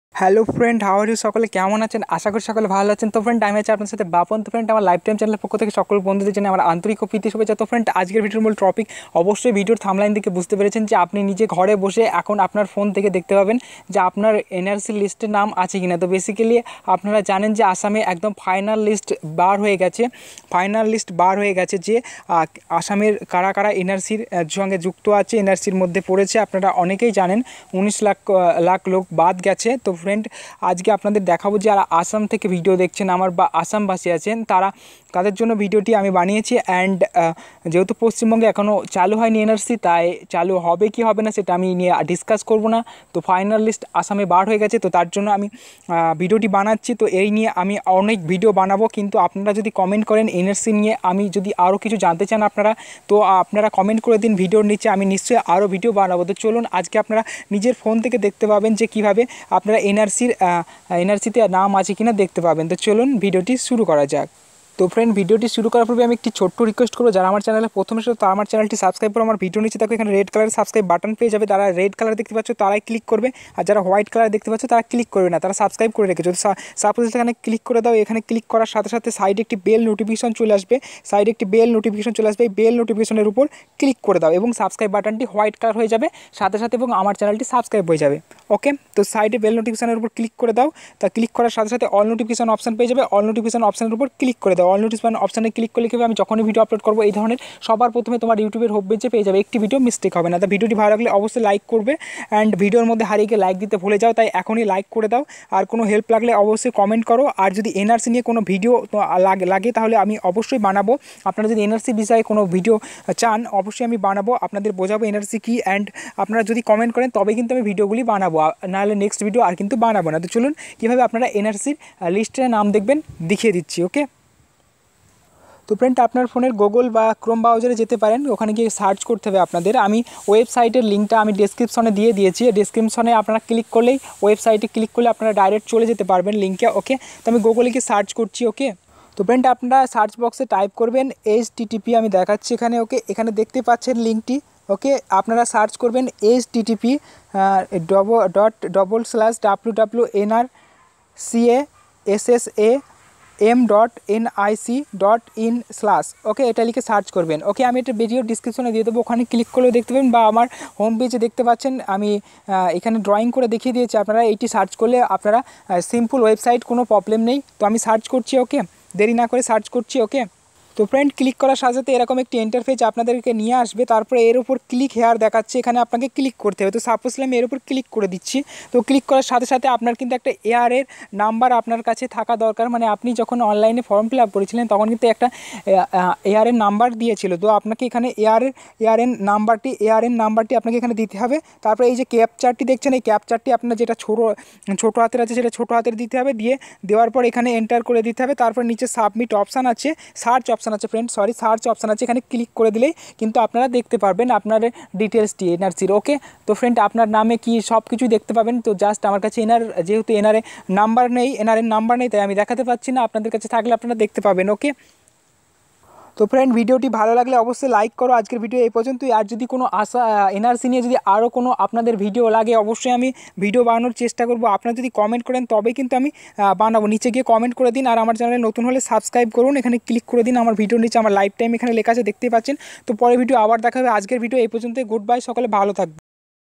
NRC List Name - 2019 NRCআপনার নাম আছে কিনা নিজের মোবাইল থেকে দেখে নিন।
NRC List Name - 2019 NRC তে আপনার নাম আছে কিনা নিজের মোবাইল থেকে দেখে নিন।